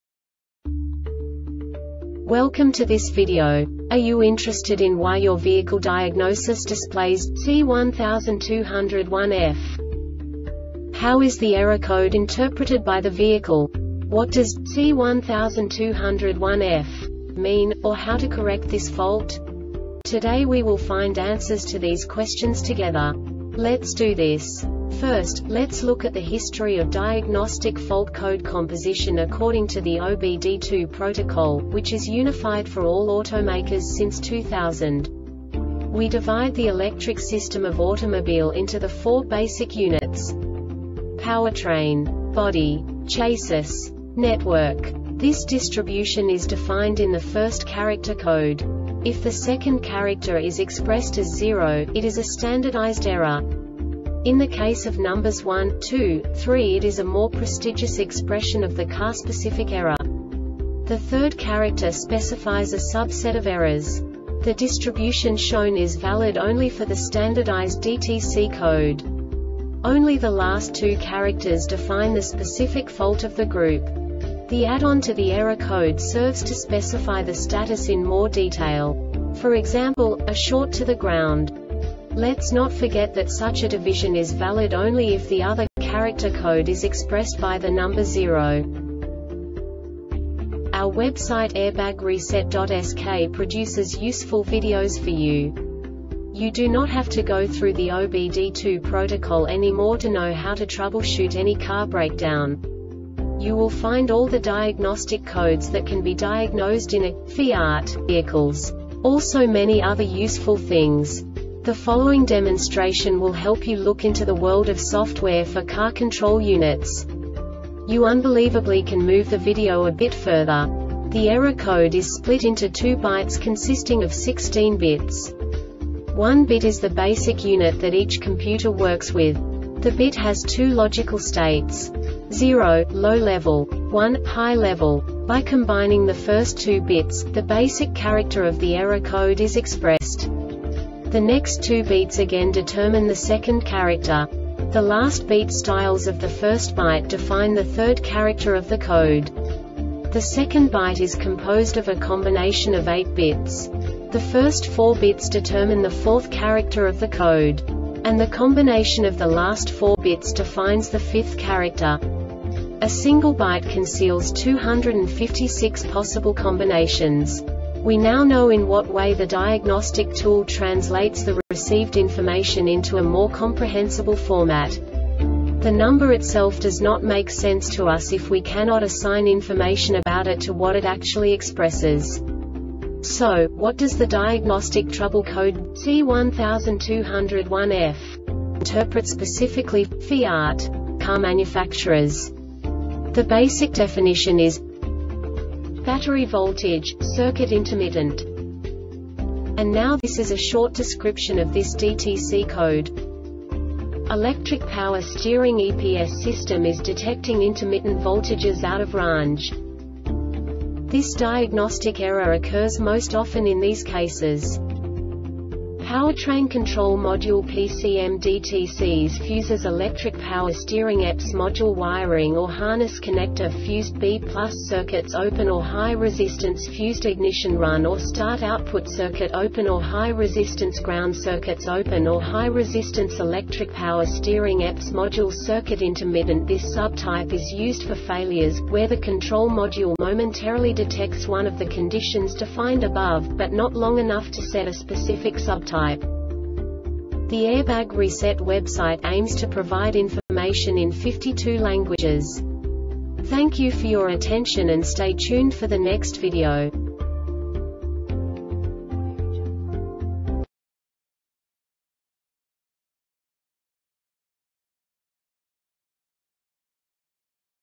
DTC Fiat C1200-1F Short Explanation
Contents:
0:21 Basic DTC analysis according to OBD2 protocol standard.
1:48 Insight into programming
2:58 A diagnostically understandable description of the Diagnostic Trouble Code
3:05 Basic error definition

Make
FIAT
Code
C1200-1F
Definition
Battery Voltage - Circuit Intermittent
Description
Electric Power Steering EPS System is detecting intermittent voltages out of raange
Cause
Powertrain Control Module Pcm Dtcs
Fuses
Electric Power Steering Eps Module Wiring or Harness Connector
Fused B+ Circuits Open Or High Resistance
Fused Ignition Run or Start Output Circuit Open Or High Resistance
Ground Circuits Open Or High Resistance
Electric Power Steering Eps Module
Failure Type
Circuit Intermittent
This sub type is used for failures, where the control module momentarily detects one of the conditions defined above, but not long enough to set a specific sub type